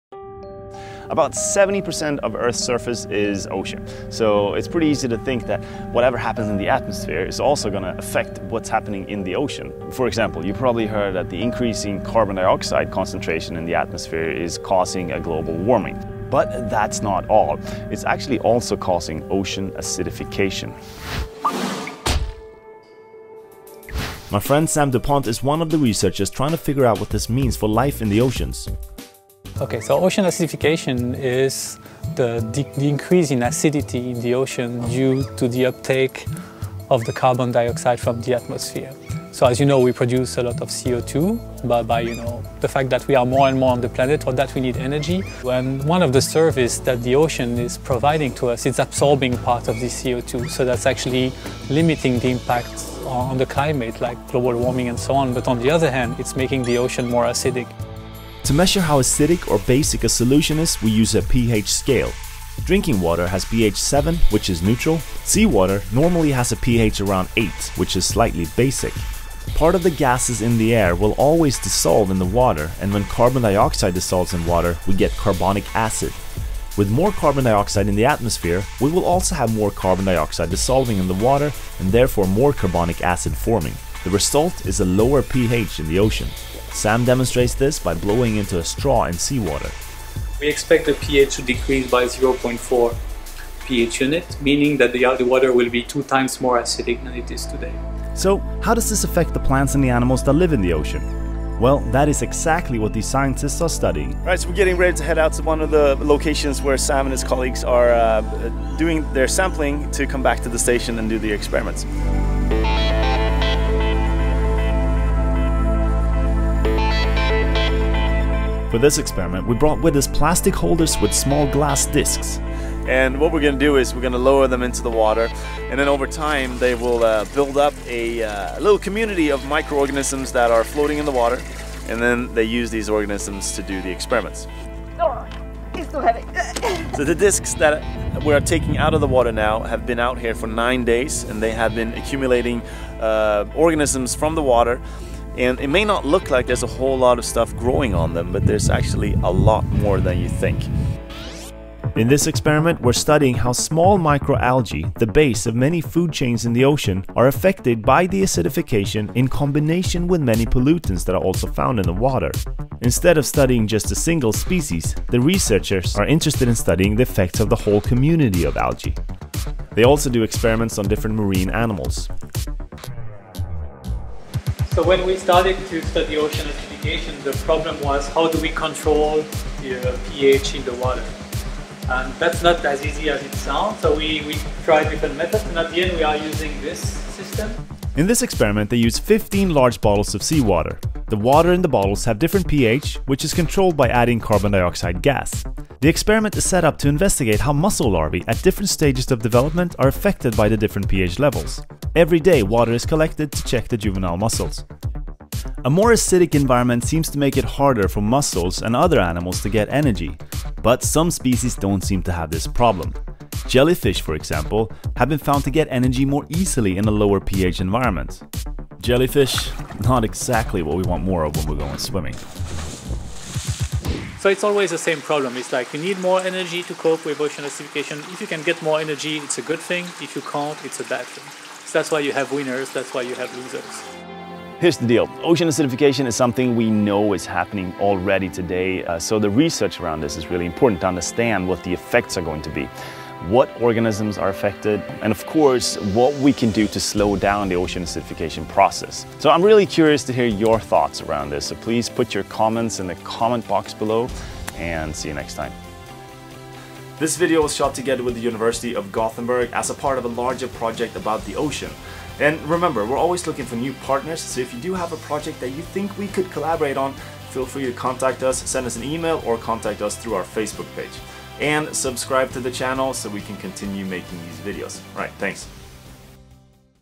Is the ocean turning sour?
Why are the oceans going sour? ... and more, explained!

Did you know that increasing carbon dioxide in the atmosphere could make prawns in the ocean taste bad? Scientists have done experiments and figured out that prawns living in slightly more acidic (lower pH) water were not as tasty as prawns living in seawater with a normal pH. Increasing CO2 levels in the atmosphere are making the ocean more acidic. And this is unfortunately something that could have an effect on a lot more than our shrimp salads.